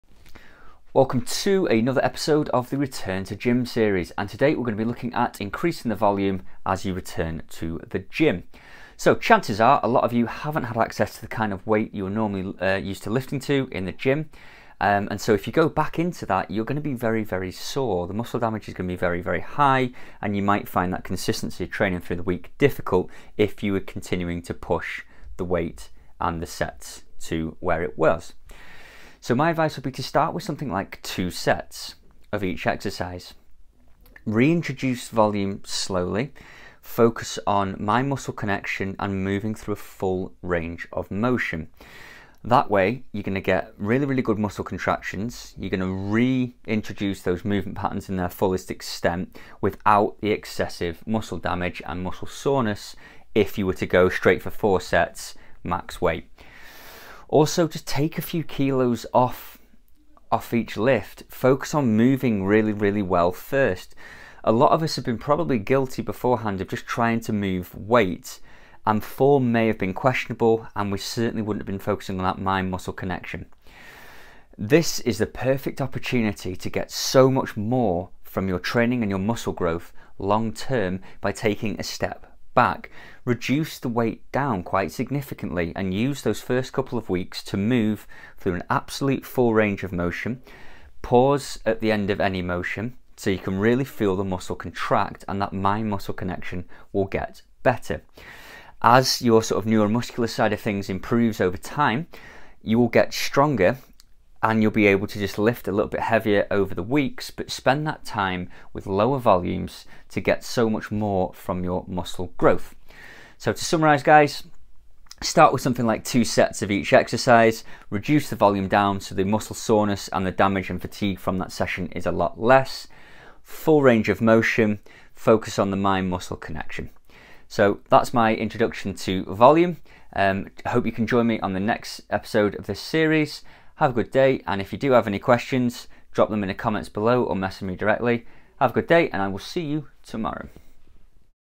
Return to the gym series - Training volume
When we all get access to gyms and for many of us much more weight it will be a very smart move to control your training volume to reduce the muscle soreness, damage and fatigue that these sessions will have.

When reintroducing the heavier weight training I would advise that you start with 2 sets of exercises, reduce the amount of total work done in a session to help you ease back into full training.

I strongly recommend that you take a good amount of weight off the bar/dumbbell/machine, use this reintroduction period to maintain the focus on full range of motion and pausing to really hit the muscle contraction and massively improve the muscle growth.

Many of us are guilty in the past of adding weight and not really feeling the muscles contract or even reducing the range of motion to boost our egos, we have all done it!

Take a step back to massively improve your muscle growth and you will find that ultimately you will end up stronger in the long term because you have actually been improving your form and building muscle properly.